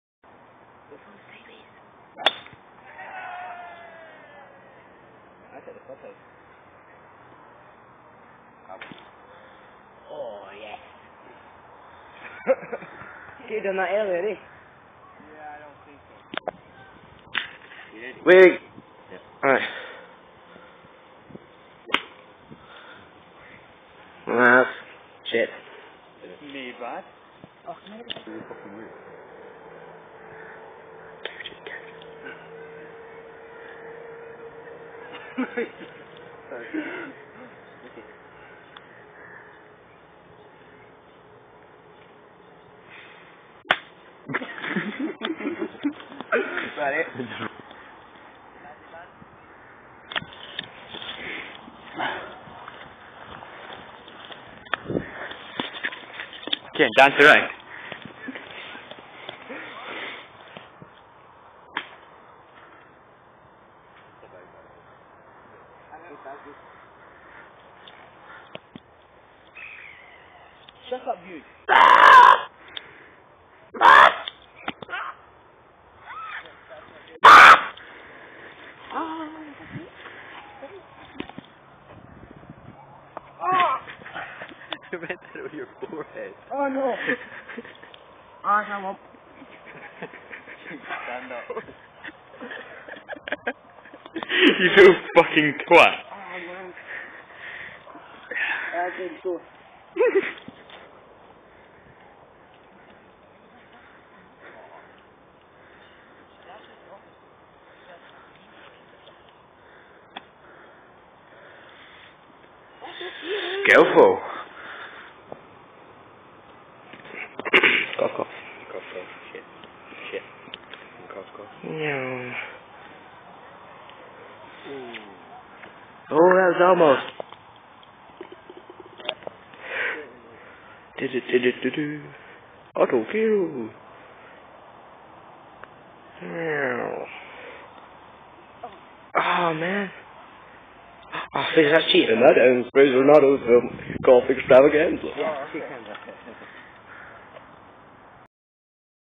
Fraser and Otto playing golf hole 18 at Linlithgow golf club
Fraser wins :(
me and gavin being assholes